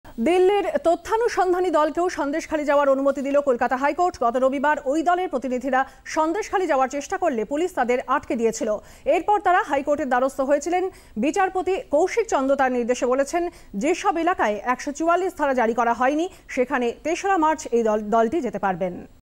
দিল্লির তথ্যানুসন্ধানীদলকে অনুমতি কলকাতা হাইকোর্টের
দিল্লির তথ্যানুসন্ধানীদলকে সন্দেশখালি যাওয়ার অনুমতি দিল কলকাতা হাইকোর্ট।